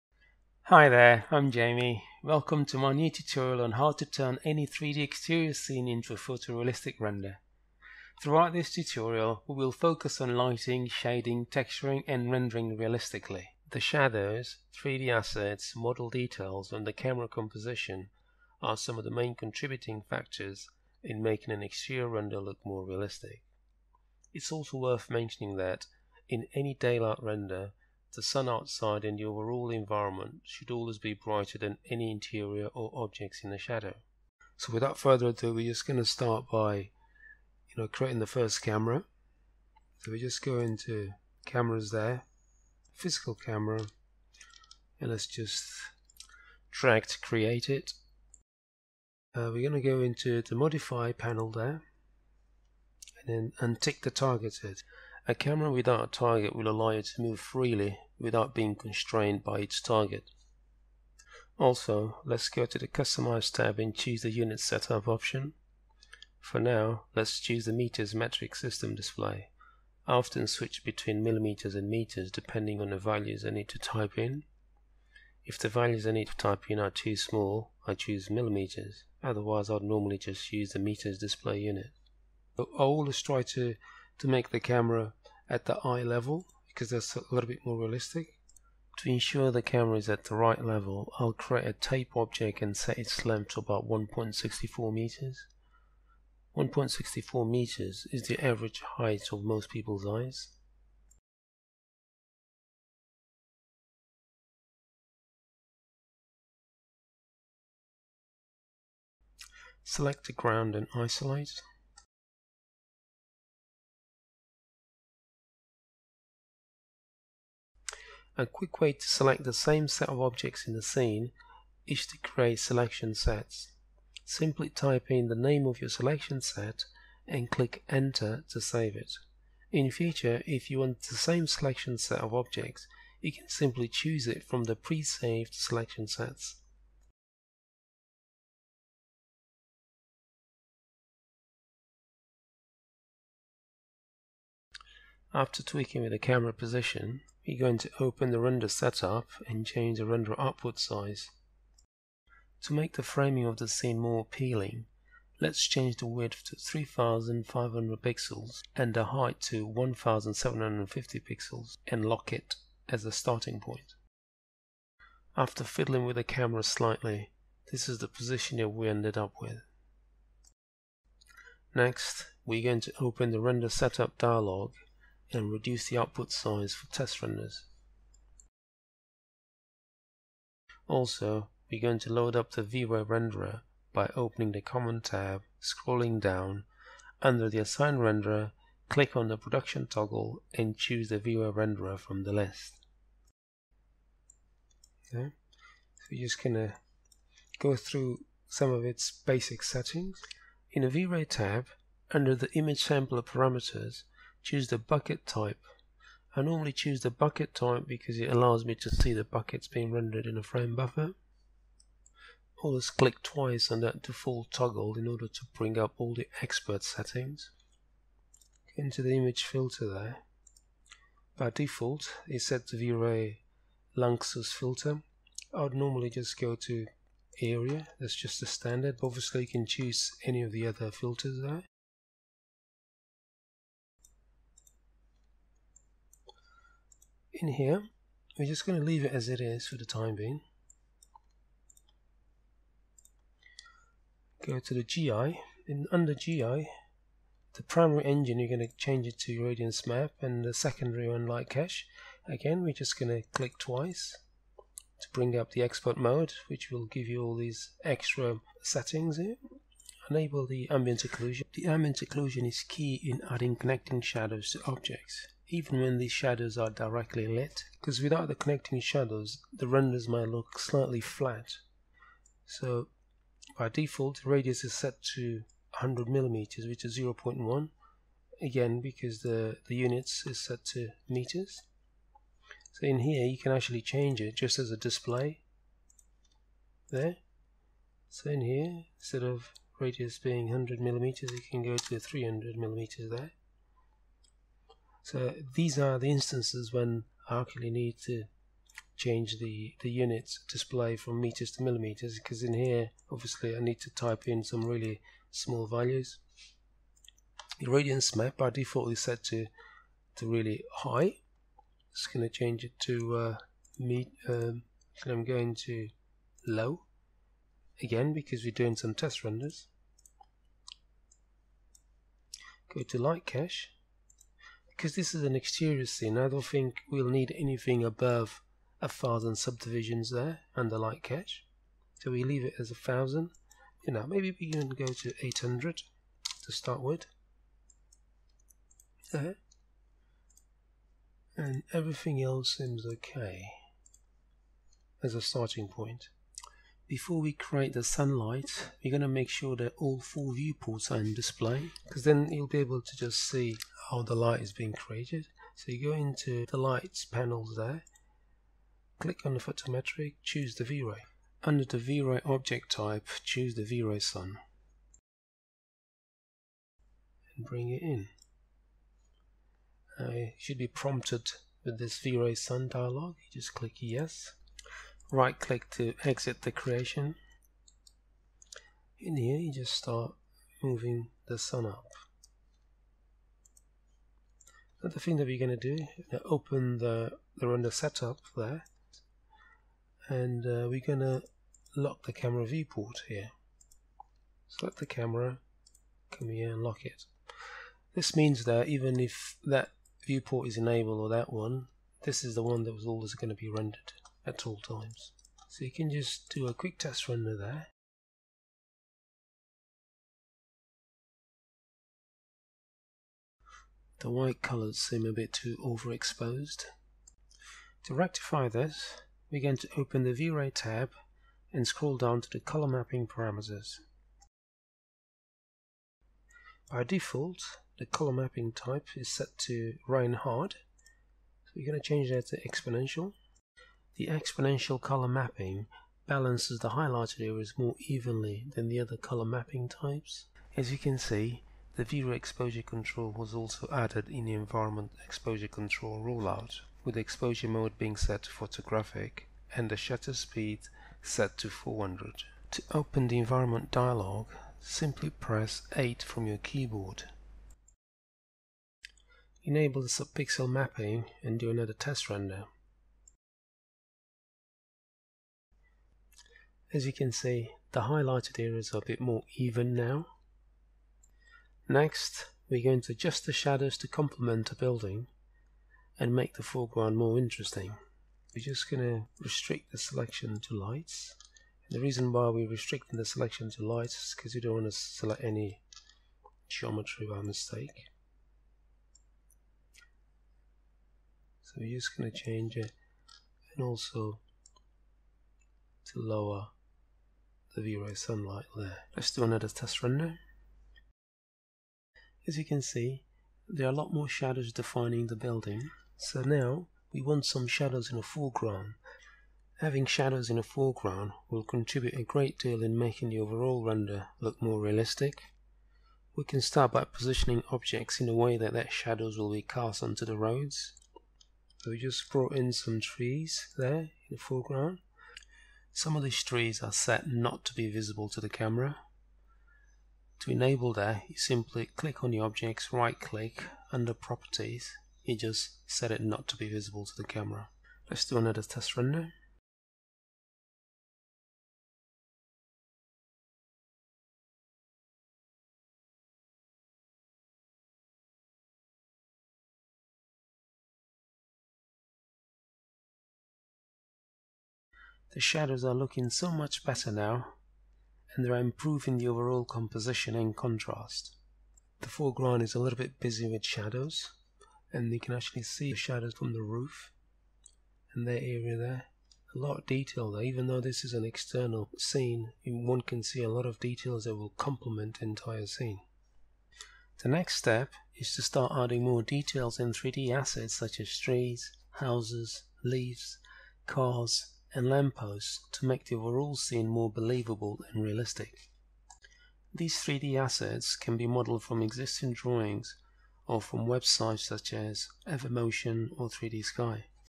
3d rendering : architectural visualization with Vray & 3ds Max Part 1(CC)
This 10 minutes clip takes users through the amazing process of lighting and rendering external daylight shot.
The remaining 5 parts will take users through the process of Shading, Texturing, Rendering, and the PostProduction of Photorealistic Day & Night scenes; while using 3ds Max, Vray and Photoshop (similar post-production techniques can also be used in After Effects) .
My methodologies are widely implemented by Award Winning Visualization companies and famous Film Studios World Wide.
Who this course is for:

    Designers
    Architects
    3d Artists
    3D Visualizers
    Anyone interested in Mastering their Shading, Texturing, Lighting, Rendering, Post-Production Skills, Etc